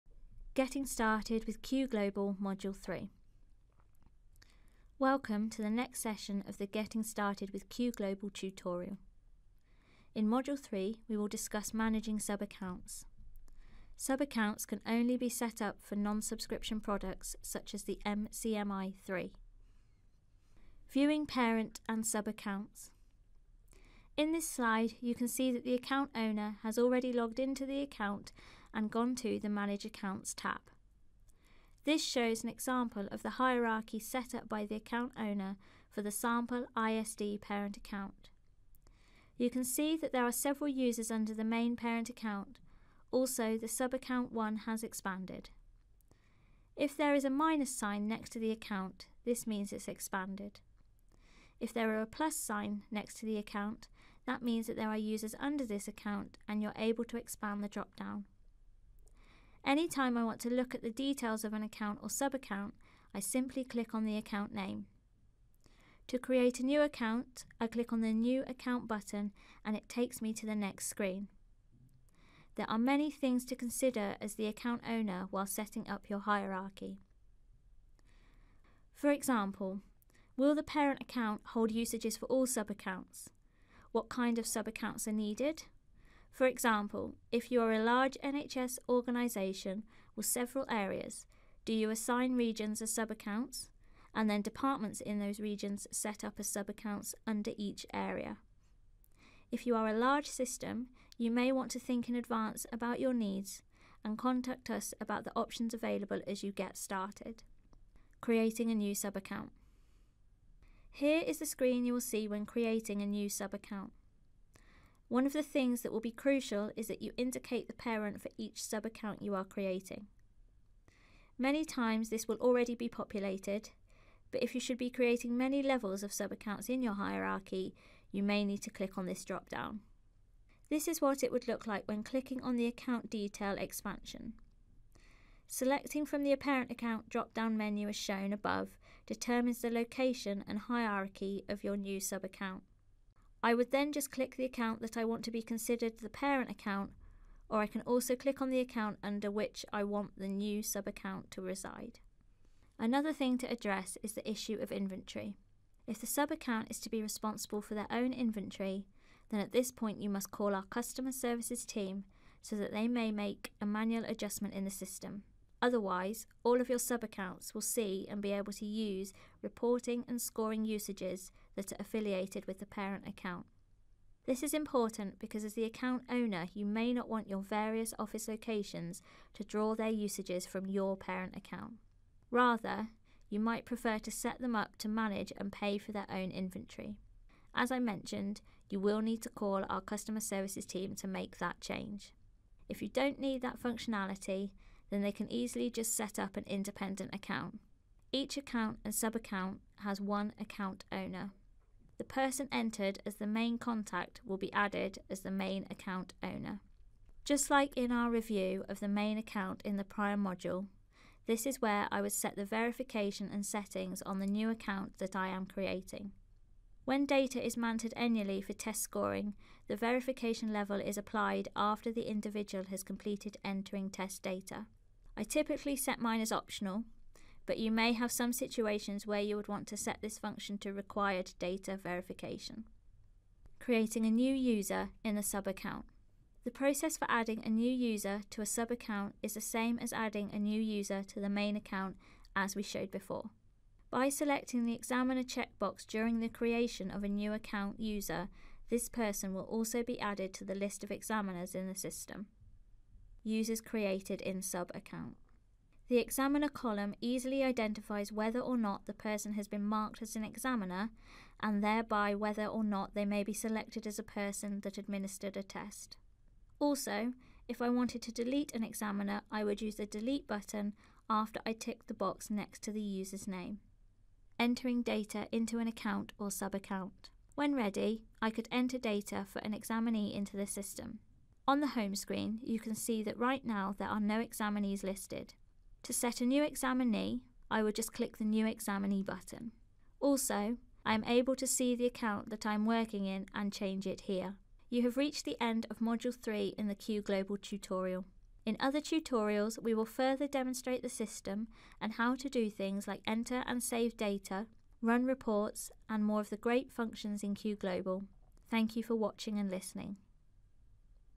Getting started with Q-global Module 3
Getting started with Q-global, Module 3: Managing Sub-Accounts

In just over five minutes learn how to use Q-global to manage sub-accounts available for non-subscription products such as the MCMI.